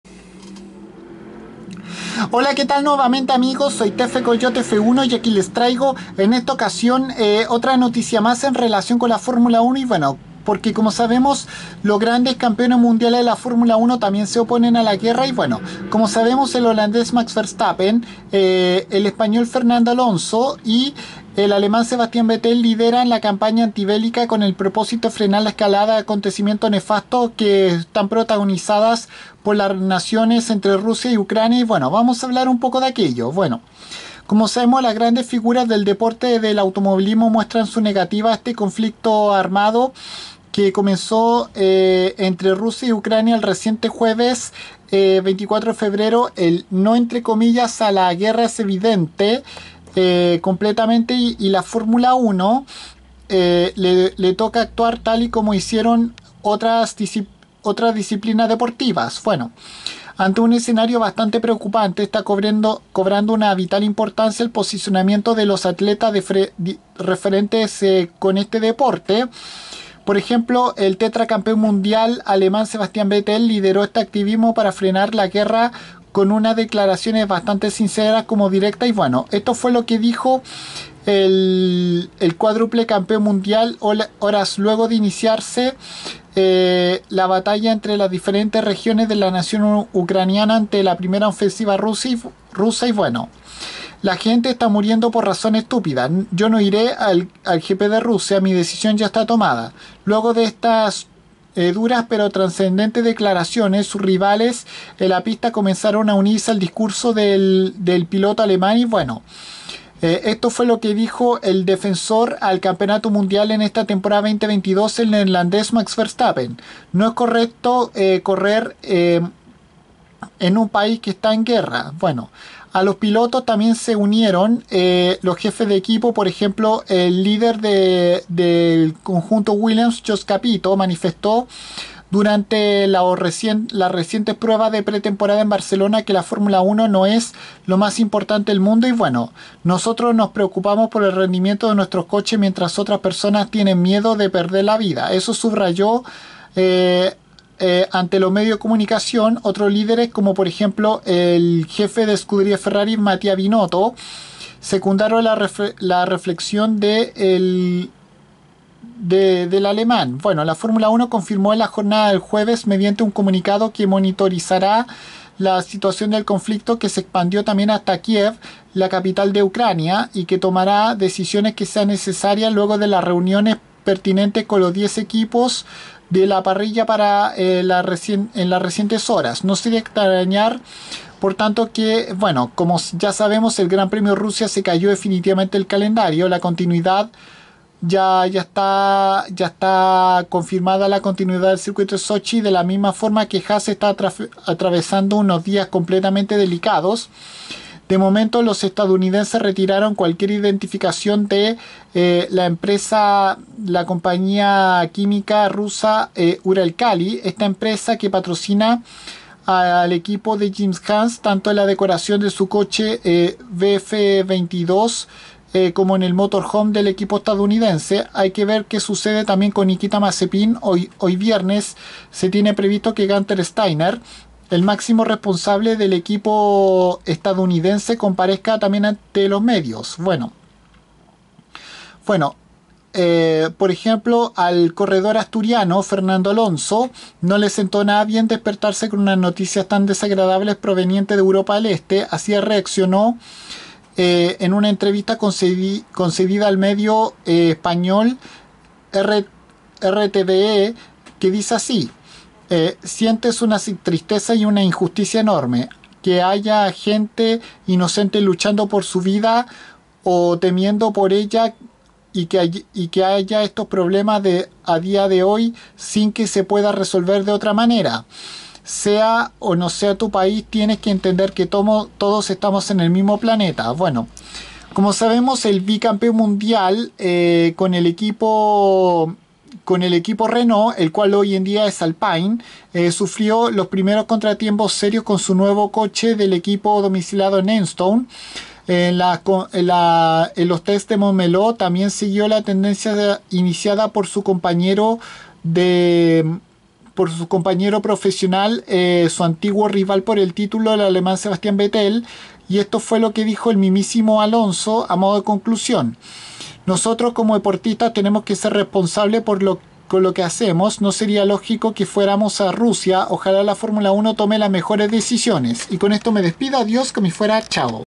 Los grandes campeones de la F1 también están en contra de la guerra
Las grandes figuras como el español Fernando Alonso el alemán Sebastian Vettel o el actual defensor de campeonato el neerlandés Max Verstappen tampoco apoyan al gobierno ruso por invadir y destruir el territorio ucraniano